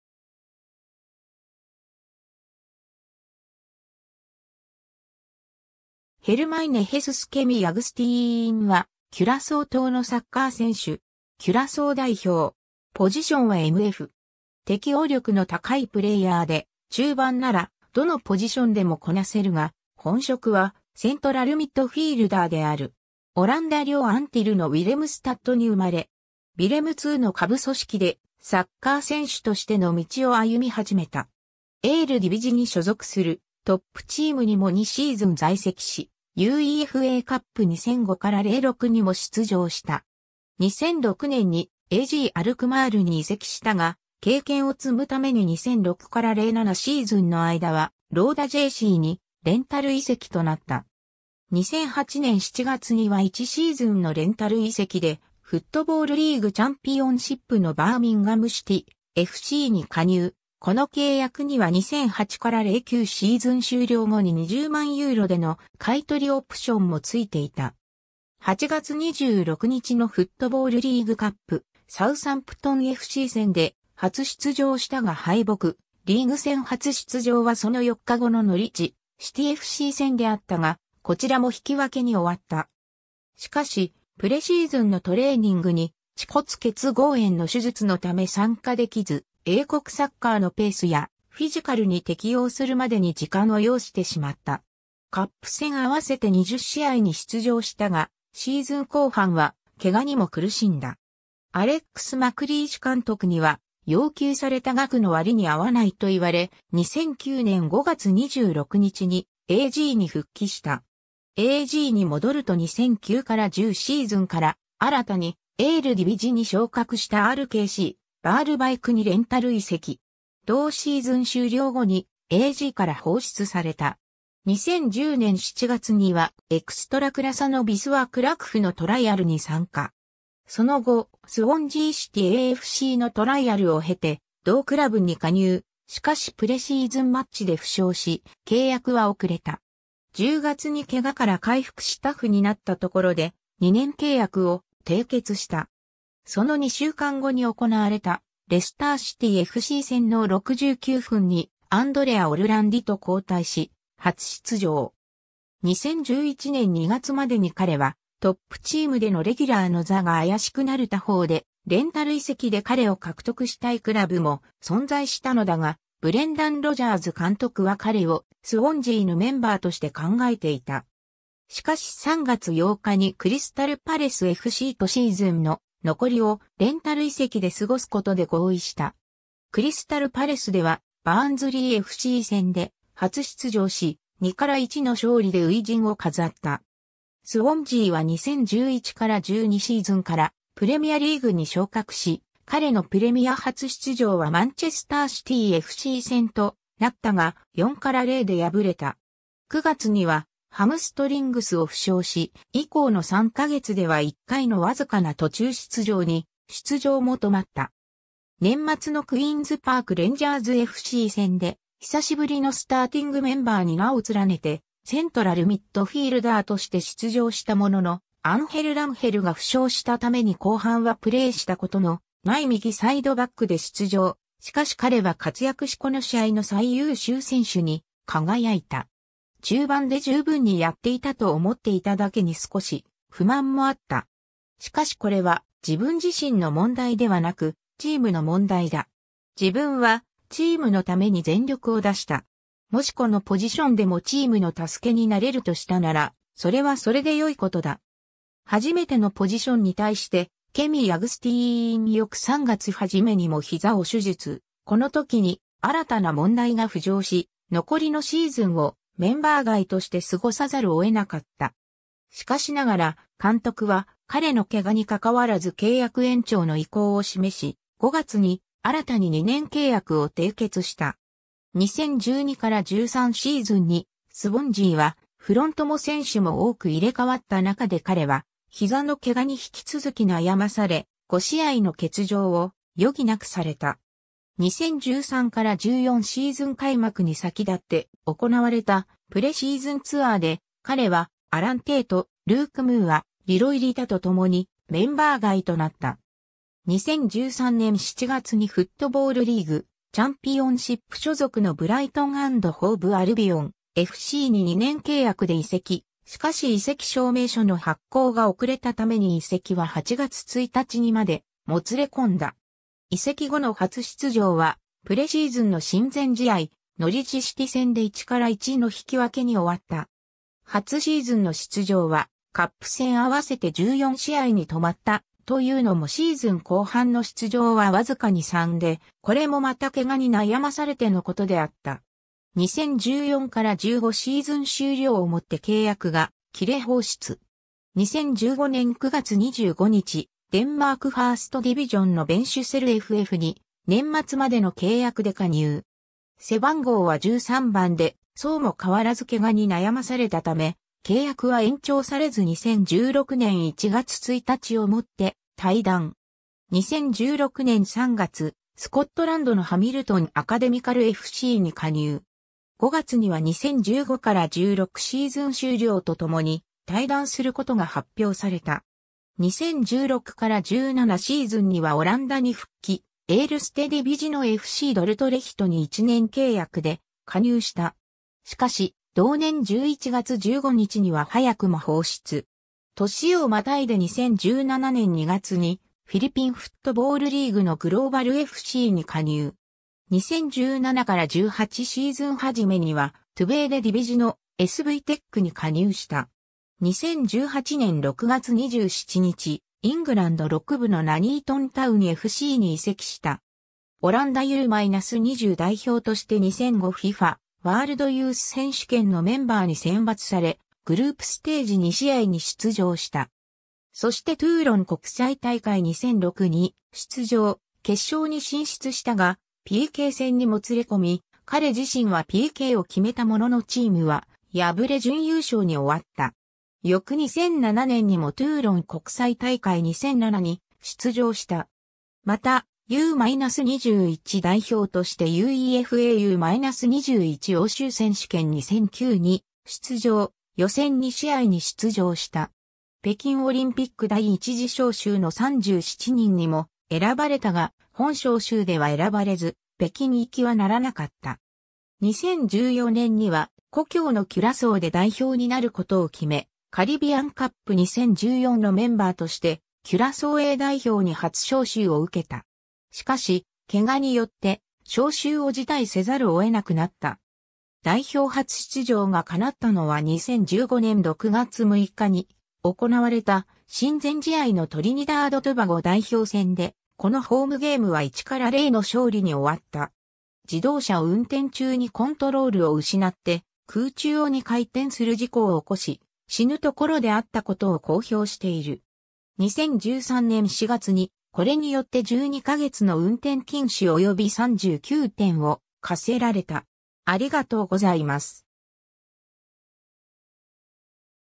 ケミー・アグスティーン
バーミンガム・シティFCの選手
スウォンジー・シティAFCの選手
ヘルマイネ・ヘスス・"ケミー"・アグスティーン（オランダ語: Germaine Hesus "Kemy" Agustien、1986年8月20日 - ）は、キュラソー島のサッカー選手。
キュラソー代表。
 適応力の高いプレーヤーで、中盤ならどのポジションでもこなせるが、本職はセントラルミッドフィールダーである。
 オランダ領アンティルのウィレムスタットに生まれ。
ヴィレムIIの下部組織でサッカー選手としての道を歩み始めた。
エールディヴィジに所属するトップチームにも2シーズン在籍し、UEFAカップ 2005-06にも出場した。
2006年にAZアルクマールに移籍したが、経験を積むために2006-07シーズンの間はローダJCにレンタル移籍となった。
 2008年7月には1シーズンのレンタル移籍でフットボールリーグ・チャンピオンシップのバーミンガム・シティFCに加入、この契約には2008-09シーズン終了後に20万ユーロでの買取オプションもついていた。
8月26日のフットボールリーグカップ、サウサンプトンFC戦で初出場したが敗北、リーグ戦初出場はその4日後のノリッジ・シティFC戦であったがこちらも引分に終わった。
しかし、プレシーズンのトレーニングに恥骨結合炎の手術のため参加できず、英国サッカーのペースやフィジカルに適応するまでに時間を要してしまった。
カップ戦合わせて20試合に出場したが、シーズン後半は怪我にも苦しんだ。
アレックス・マクリーシュ監督には「要求された額の割に合わない」と言われ、2009年5月26日にAZに復帰した。
AZに戻ると2009-10シーズンから新たにエールディヴィジに昇格したRKCヴァールヴァイクにレンタル移籍。
同シーズン終了後にAZから放出された。
 2010年7月にはエクストラクラサのヴィスワ・クラクフのトライアルに参加。
その後、スウォンジー・シティAFCのトライアルを経て同クラブに加入、しかしプレシーズンマッチで負傷し契約は遅れた。
10月に怪我から回復しタフになった所で、2年契約を締結した。
その2週間後に行われたレスター・シティFC戦の69分にアンドレア・オルランディと交代し、初出場。
2011年2月までに彼はトップチームでのレギュラーの座が怪しくなる他方で、レンタル移籍で彼を獲得したいクラブも存在したのだが、ブレンダン・ロジャーズ監督は彼をスウォンジーのメンバーとして考えていた。
しかし3月8日にクリスタル・パレスFCとシーズンの残りをレンタル移籍で過ごす事で合意した。
クリスタル・パレスではバーンズリーFC戦で初出場し、2-1の勝利で初陣を飾った。
 スウォンジーは2011-12シーズンからプレミアリーグに昇格し、彼のプレミア初出場はマンチェスター・シティFC戦となったが、4-0で敗れた。
9月にはハムストリングスを負傷し、以降の3か月では1回のわずかな途中出場に出場も止まった。
年末のクイーンズ・パーク・レンジャーズFC戦で久し振りのスターティングメンバーに名を連ねてセントラルミッドフィールダーとして出場したものの、アンヘル・ランヘルが負傷したために後半はプレーした事のない右サイドバックで出場、しかし彼は活躍しこの試合の最優秀選手に輝いた。
 中盤で十分にやっていたと思っていただけに少し不満もあった。
しかしこれは自分自身の問題ではなく、チームの問題だ。
自分はチームのために全力を出した。
もしこのポジションでもチームの助けになれるとしたなら、それはそれで良い事だ。
 —初めてのポジションに対して、ケミー・アグスティーン 翌3月初めにも膝を手術、この時に新たな問題が浮上し、残りのシーズンをメンバー外として過ごさざるを得なかった。
しかしながら監督は彼の怪我に関らず契約延長の意向を示し、5月に新たに2年契約を締結した。
2012-13シーズンにスウォンジーはフロントも選手も多く入れ替わった中で彼は膝の怪我に引き続き悩まされ、5試合の欠場を余儀なくされた。
 2013-14シーズン開幕に先立って行われたプレシーズンツアーで、彼はアラン・テイト、ルーク・ムーア、リロイ・リタと共にメンバー外となった。...